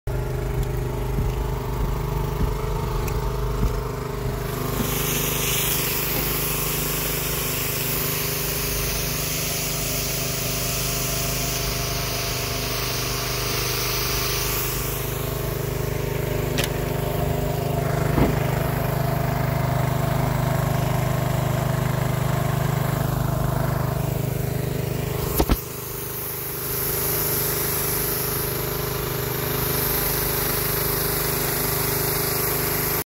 Watering the track
Yeeee, pump is kind of noisy